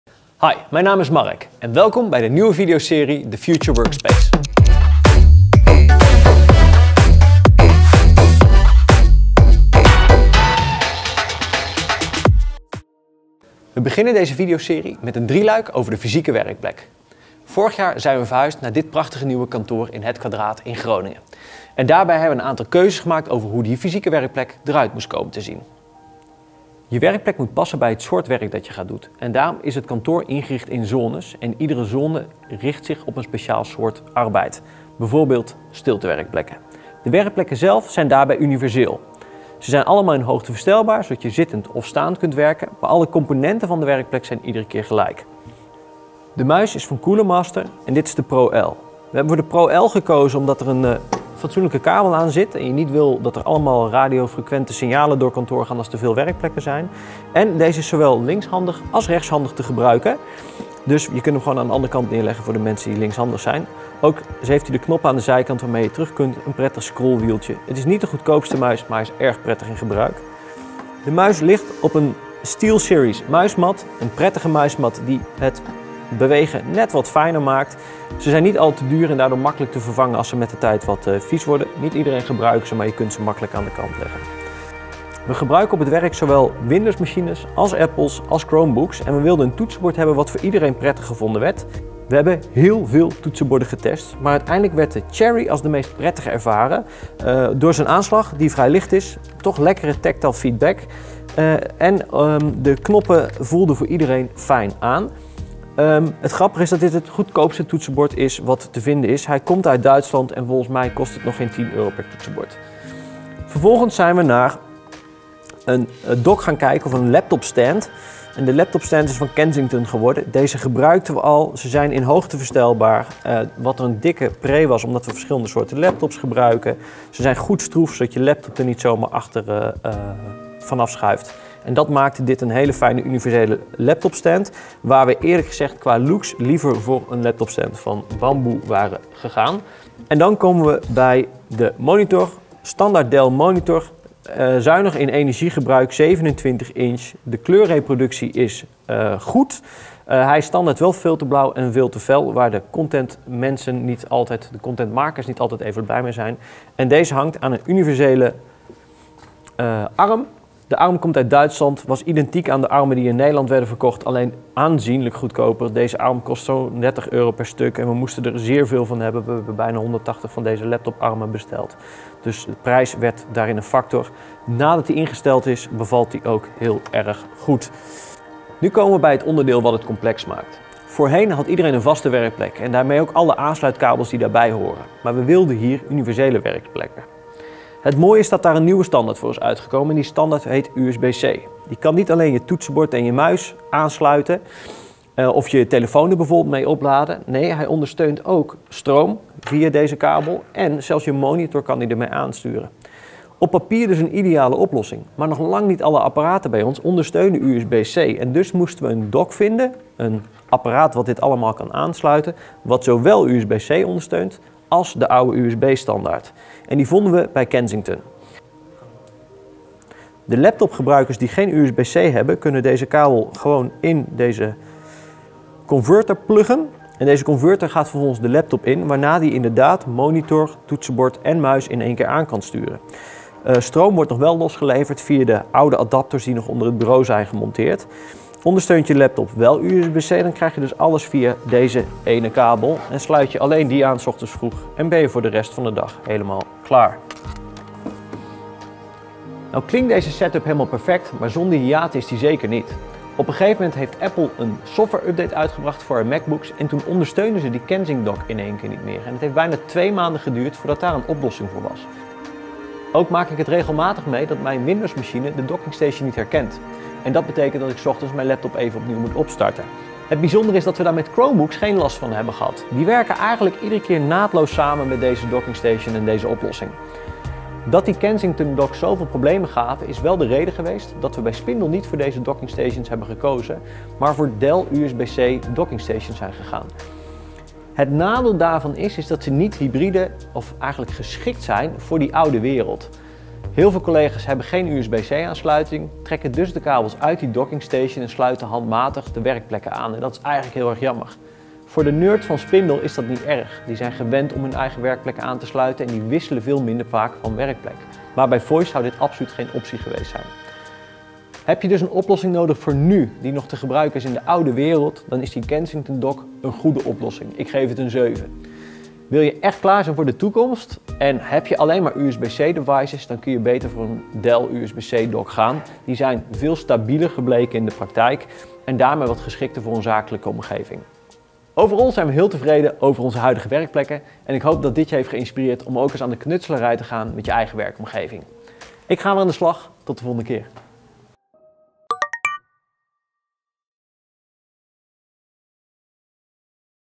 Werkplekinrichting - Handige apparaten en accessoires voor je (thuis)kantoor of universele werkplek
Voys verhuisde in 12 jaar 13x. Werkplekinrichting hebben we dus veel geoefend. Laptop, toetsenbord, USB dockingsstation. Welke apparaten en accessoires zijn handig voor een fysieke en vooral universele werkplek, welke merken kan ik aanraden en waarom? Dat vertel ik je in de eerste video uit de driedelige serie The future workspace. Dit is een werkplek die overal kan zijn: thuis én op kantoor.